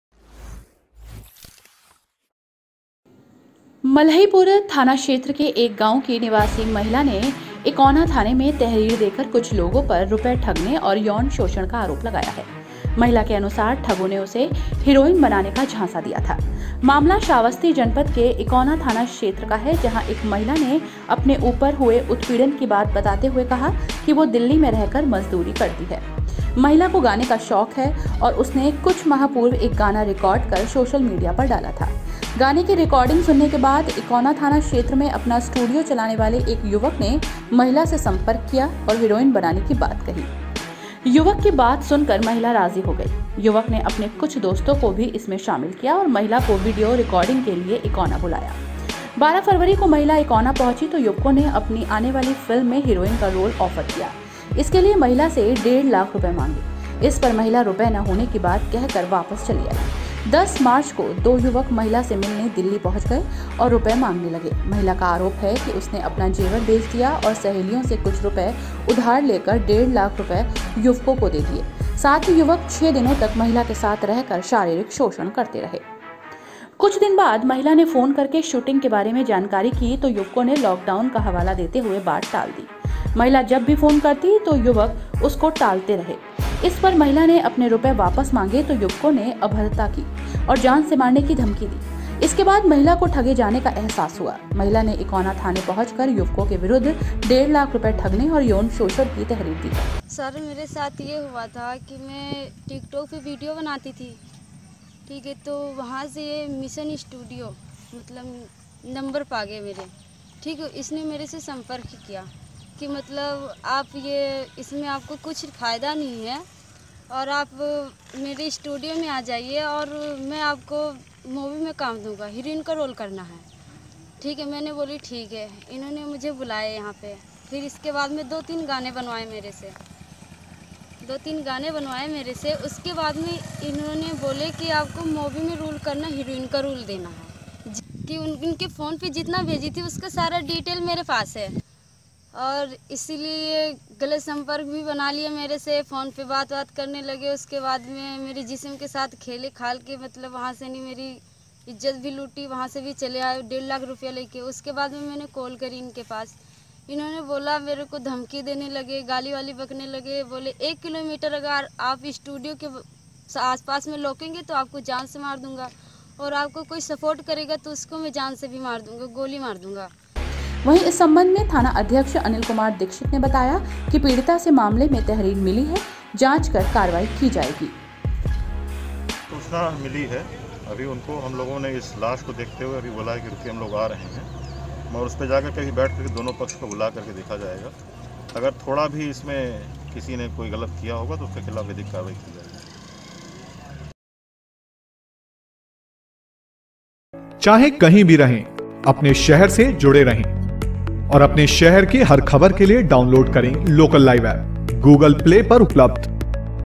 || SHRAVASTI || आंखों में हीरोइन बनने का सपना लेकर गई थी युवती हुआ ये कांड
अपने शहर की खबरों को विस्तार से देखने के लिए अभी डाउनलोड करें लोकल लाइव

मल्हीपुर थाना क्षेत्र के एक गांव की निवासी महिला ने इकौना थाने में तहरीर देकर कुछ लोगों पर रुपये ठगने और यौन शोषण का आरोप लगाया है। महिला के अनुसार ठगों ने उसे हीरोइन बनाने का झांसा दिया था।

मामला श्रावस्ती जनपद के इकौना थाना क्षेत्र का है जहां एक महिला ने अपने ऊपर हुए उत्पीड़न की बात बताते हुए कहा कि वह दिल्ली में रहकर मजदूरी करती है। महिला को गाने का शौक है और उसने कुछ माह पूर्व एक गाना रिकार्ड कर सोशल मीडिया पर डाला था। गाने की रिकार्डिंग सुनने के बाद इकौना थाना क्षेत्र में अपना स्टूडियो चलाने वाले एक युवक ने महिला से सम्पर्क किया और हीरोइन बनाने की बात कही। युवक की बात सुनकर महिला राजी हो गई। युवक ने अपने कुछ दोस्तों को भी इसमें शामिल किया और महिला को वीडियो रिकॉर्डिंग के लिए इकौना बुलाया।
The woman, resident of a village in Malipur police station area, has accused some people of cheating money and sexual exploitation by giving tahrir at Ikona police station. According to the woman, the thugs had tricked her into making him a heroine.

The case is related to the Ekona police station area of ​​Shravasti district where a woman said that she lives in Delhi and speaks about the harassment. The woman is fond of singing and recorded a song a few months ago and put it on social media. After listening to the recording of the song, a young man running his studio in Ikona police station area approached the woman and asked to make a heroine. The woman agreed to listen to the young man's talk. The young man also joined some of his friends and called the woman for a video recording.